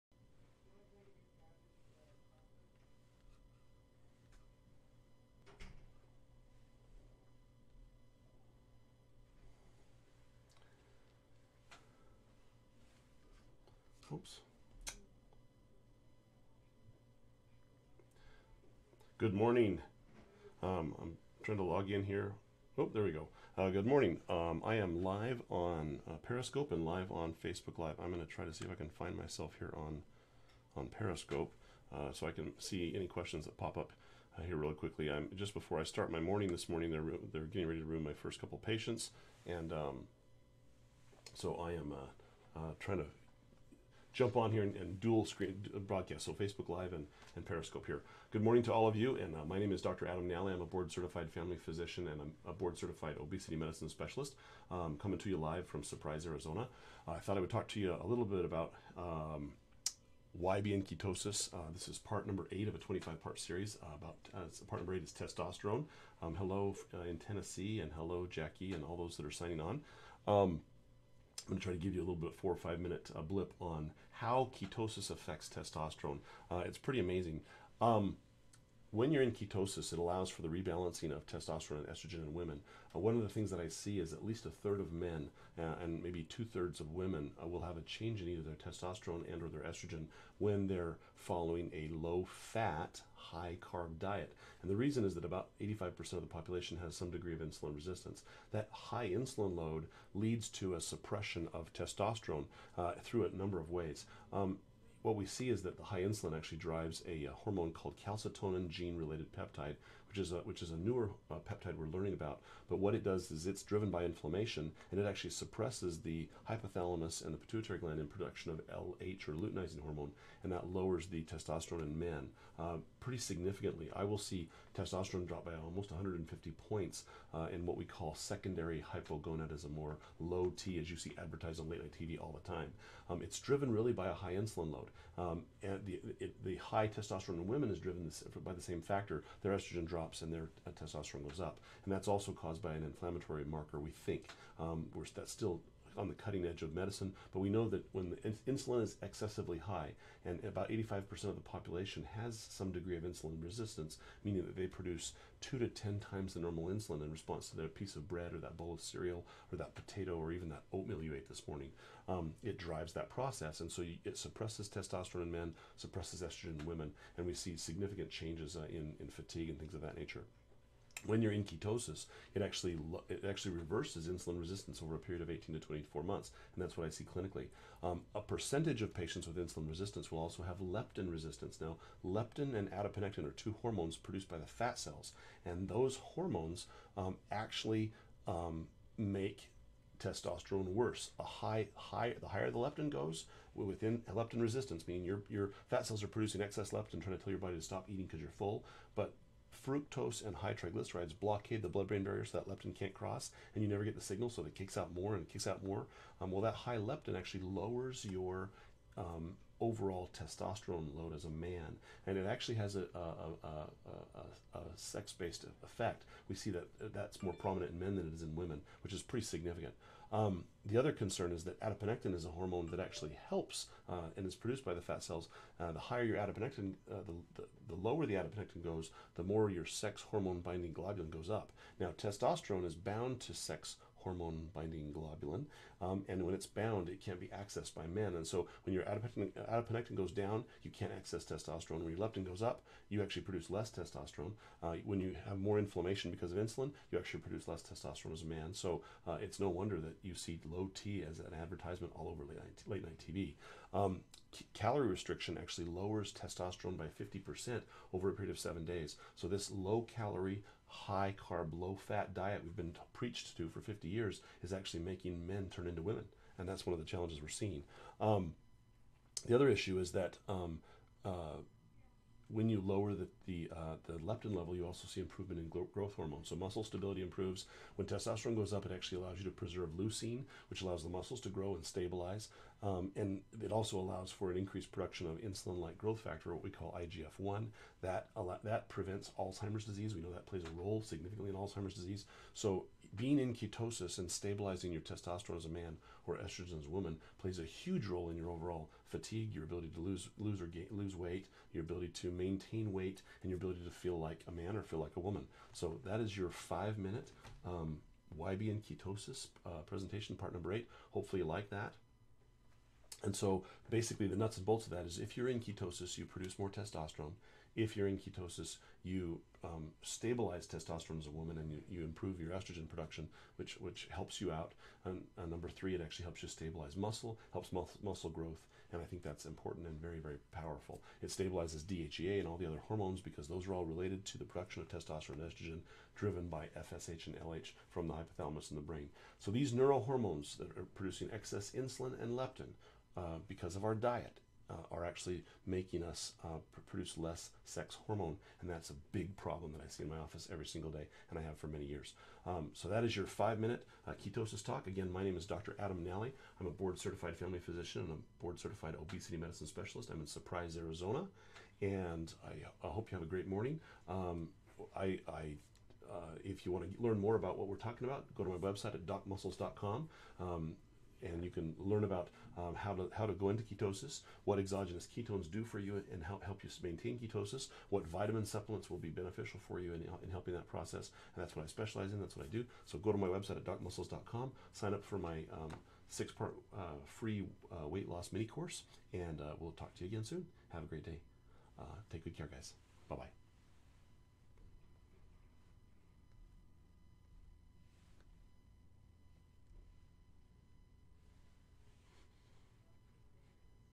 Why Be In Ketosis? Part VIII - Testosterone!!
Why Be In Ketosis? Part VIII - testosterone and why it is important to you. Find out more at DocMuscles.com and get exogenous ketones at DynamicKetones.com